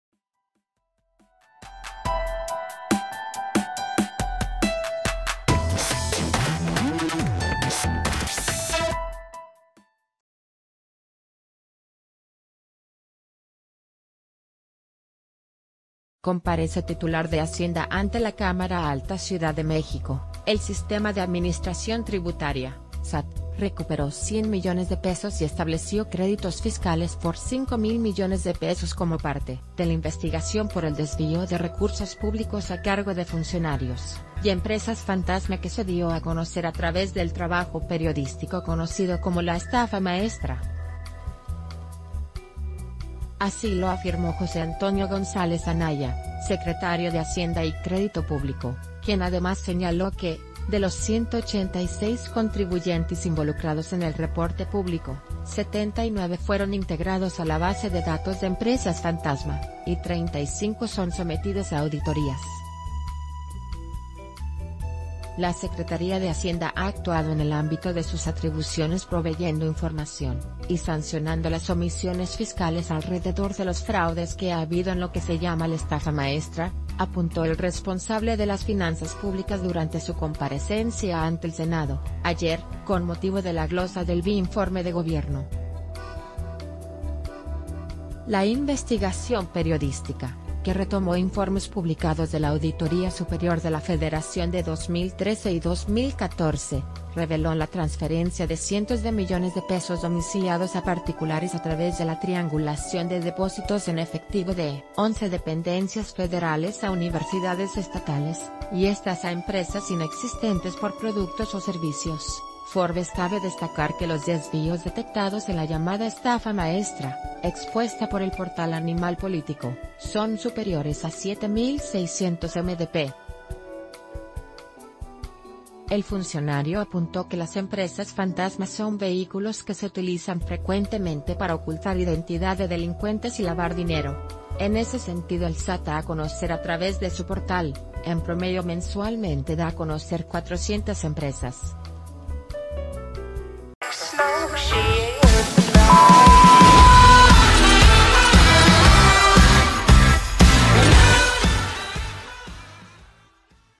El SAT recupera dinero de estafa
Comparece titular de Hacienda ante la Cámara Alta CIUDAD DE MÉXICO.— El Sistema de Administración Tributaria (SAT) recuperó 100 millones de pesos y estableció créditos fiscales por 5,000 millones de pesos como parte de la investigación por el desvío de recursos públicos a cargo de funcionarios y emp...